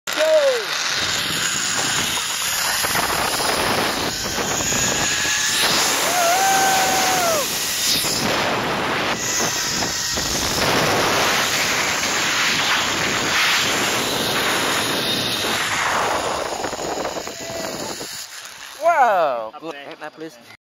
ziplining 3
ziplining with flight of the gibbons in chiang mai, thailand.  This is their longest zipline.